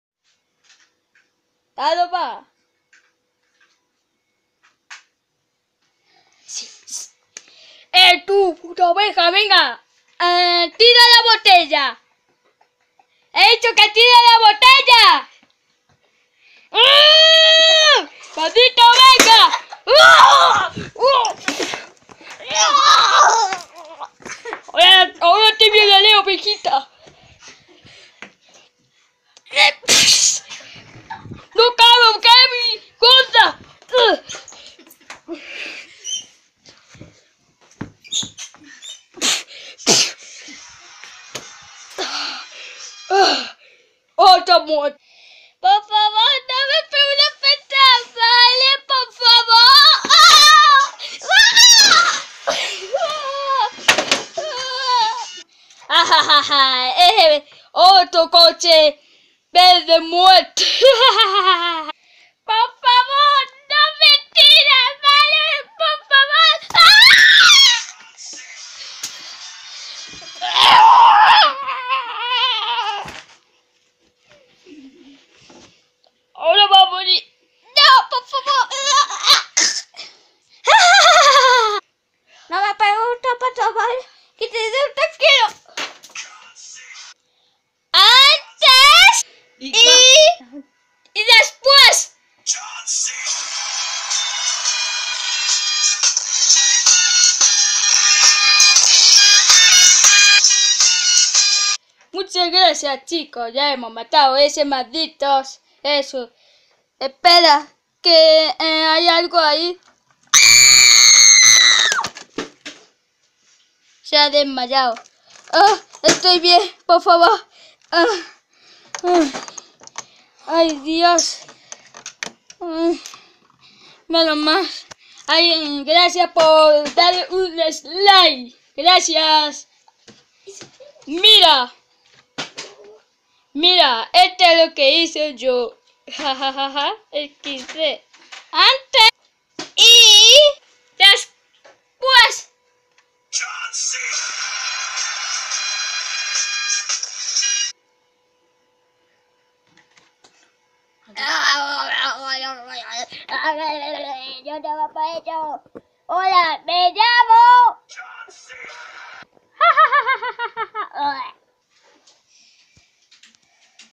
La oveja no quería tirar la botella y todos su amigos de peluche esta muertos 👻
Yo mate todos los peluches XD de la maldita oveja porque no quería y todos los peluches lo suspendieron y no quería tirar la botella del reto y Gasto la oveja 1730€...🗾😂